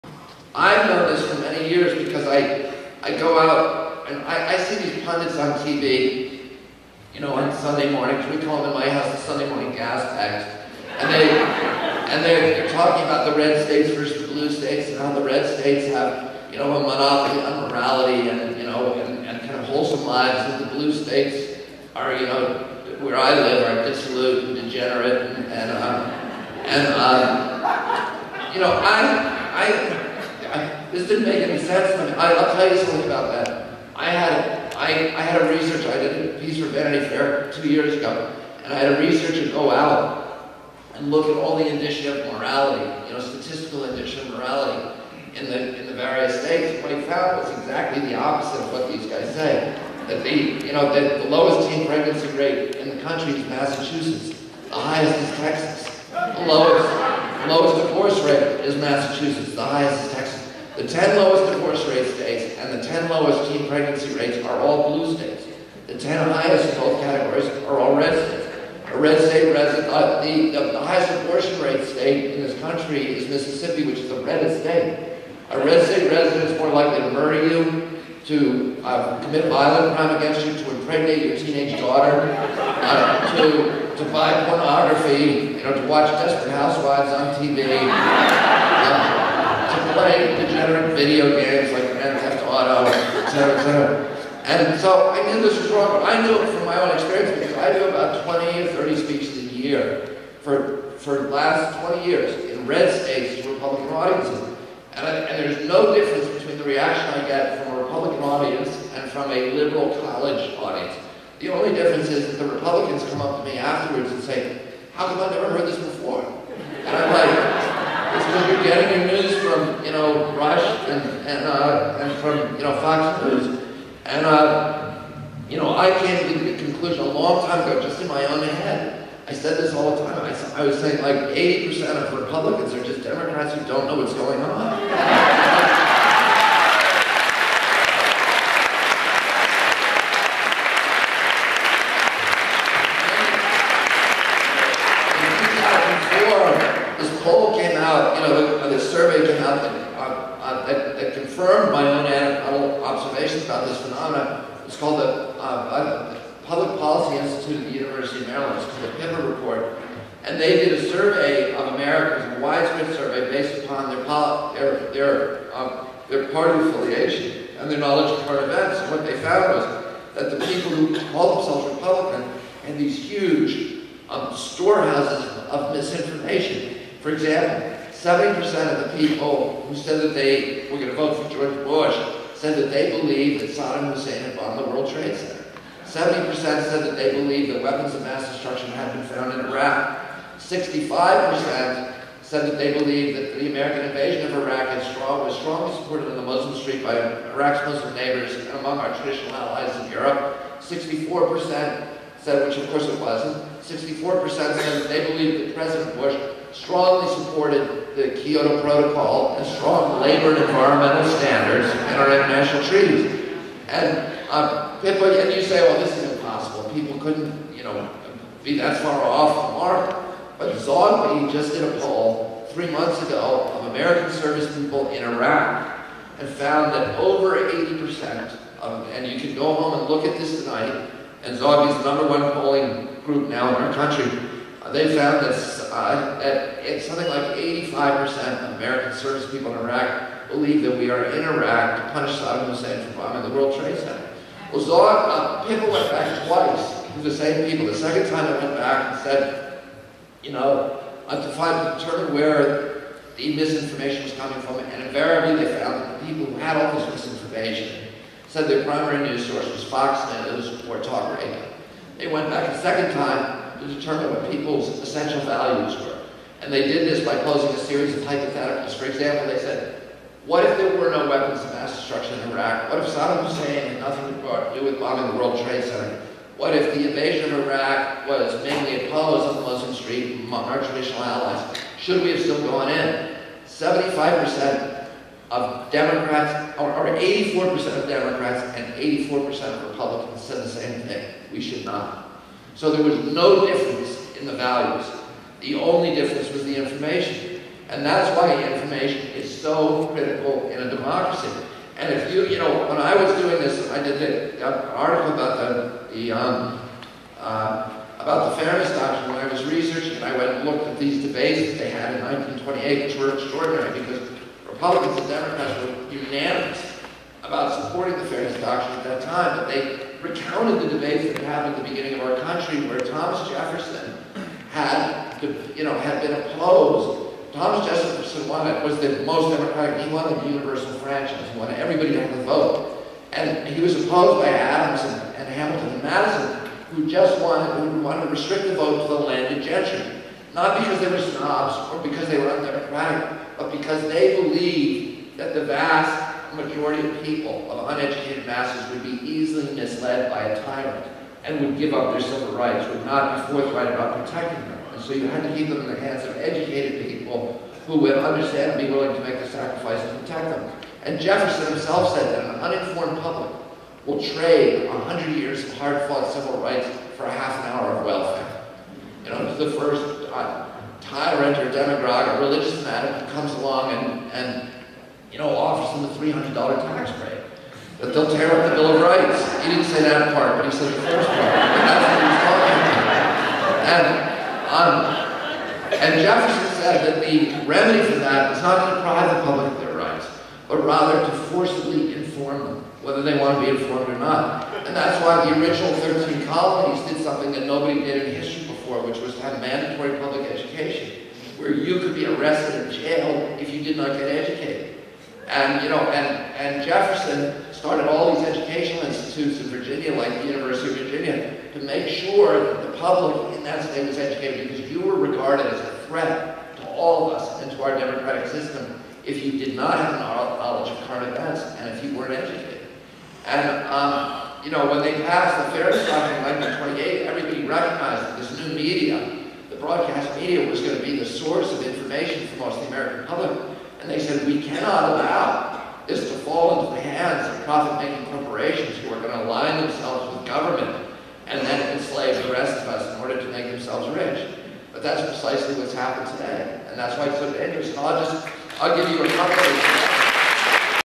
RFK Jr to Tulsa Sacred Activists PART 3 of 8
The noted son and stirring environmental activist keynotes at Wisdom U's Sacred Activism Conference, Tulsa, 5/10/07. Here: Who's good? Who's Not? Who knows? And why education is so important.